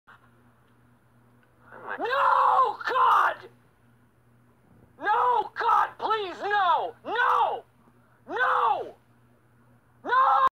My Reaction of Regular show Getting Cancelled after Season 8
I feel really sad when i heard it was coming to an end its been 6 Years in 2010 since the show frist came out but now it has to to an end why can they cancelled the rebooted shows insteas of Regular show sorry if the video is loud :(